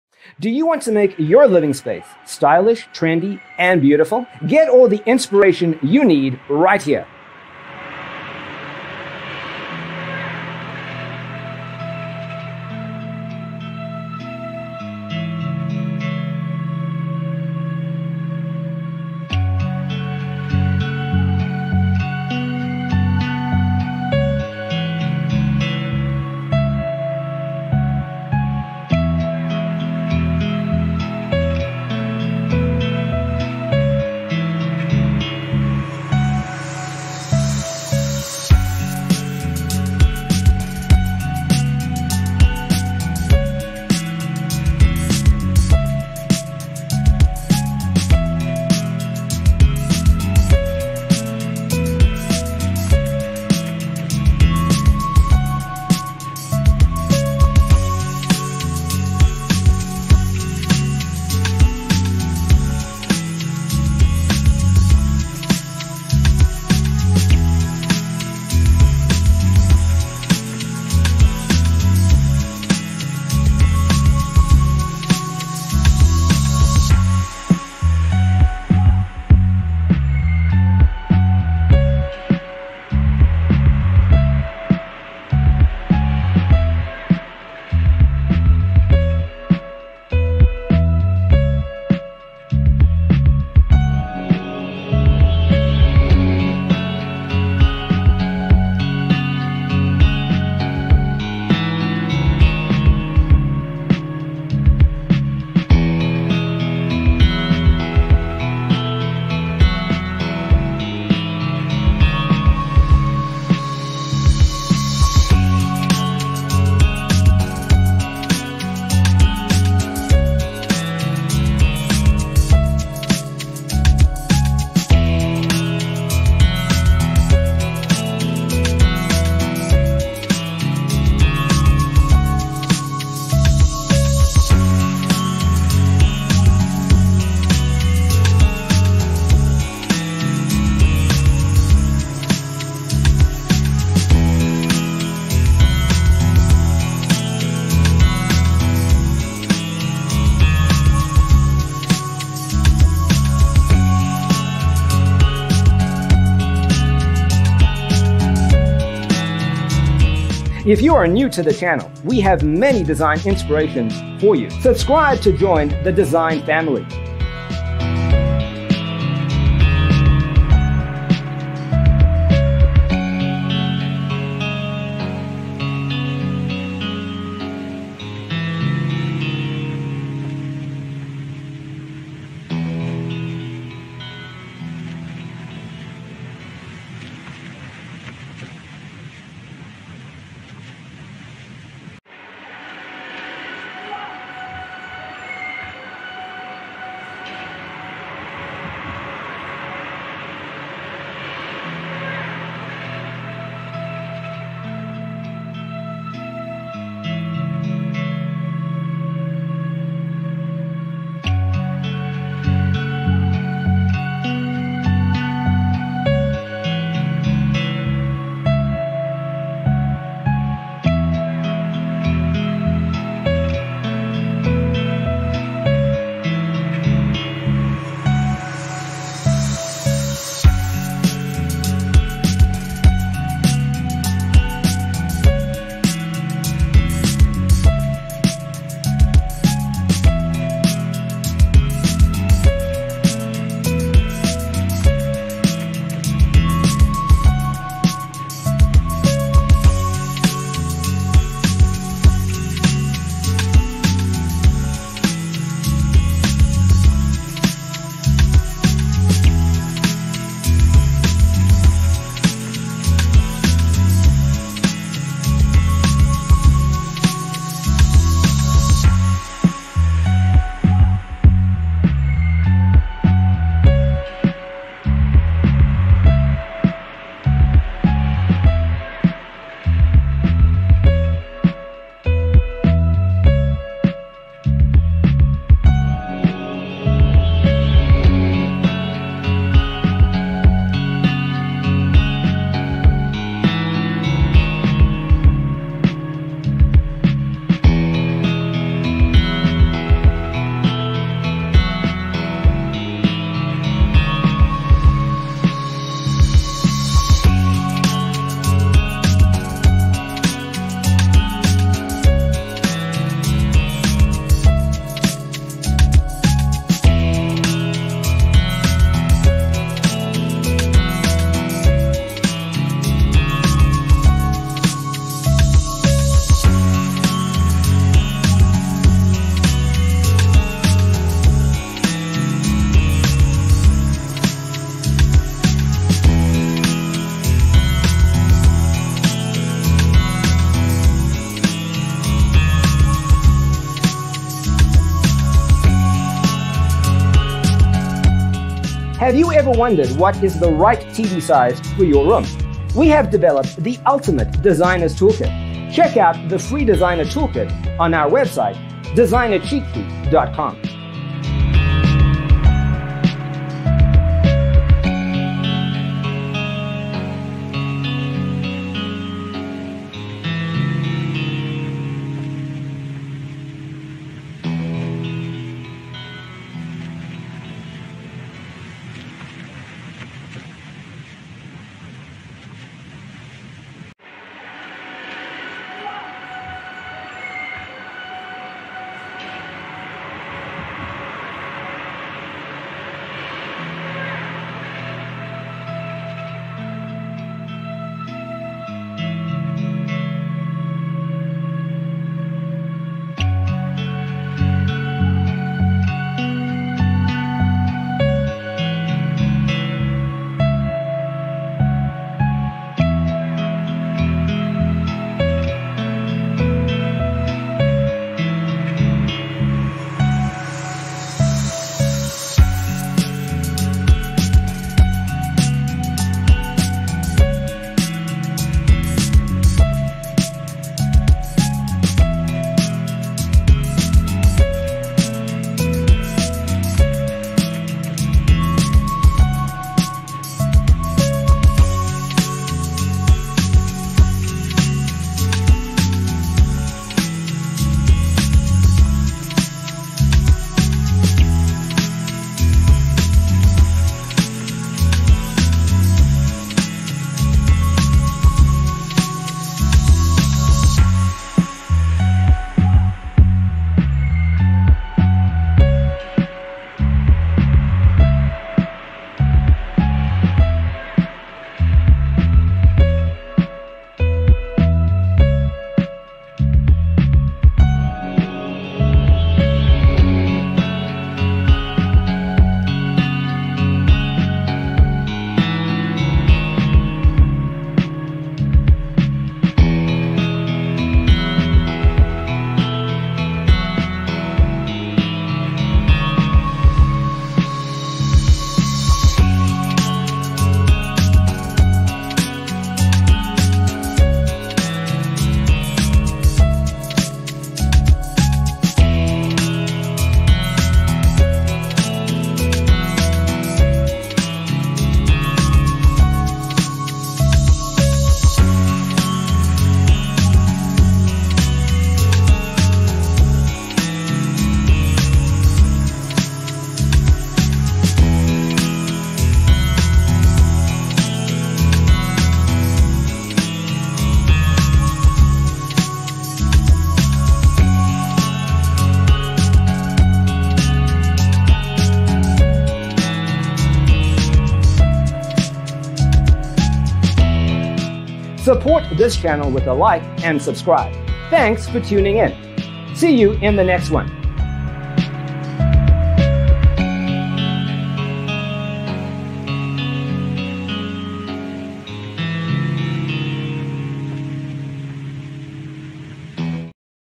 75 Modern Kitchen Design Ideas You'll Love ⭐
Browse photos of modern kitchen designs. Discover inspiration for your minimalist kitchen remodel or upgrade with ideas for storage, organization, layout and decor.

0:00:00 - Intro
0:00:12 - Modern Kitchen Design Ideas You'll Love: Inspirations 1 to 25
0:03:22 - Modern Kitchen Design Ideas You'll Love: Inspirations 26 to 50
0:06:44 - Modern Kitchen Design Ideas You'll Love: Inspirations 51 to 75

Hey hello!